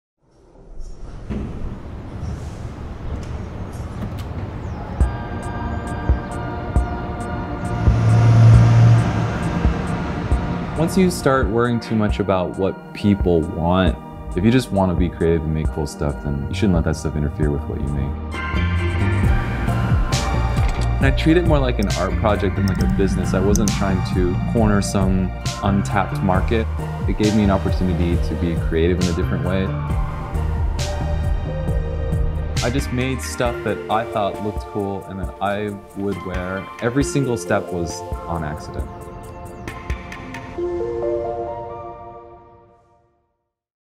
Sci-fi Fantasy x Vans Collaboration
Founded on creative self-expression, Vans leans into Jerry Hsu’s mind-melting world of Sci-Fi and the places and eras in which he draws inspiration, recently captured in a short film following his creative process, and our latest limited-edition collection.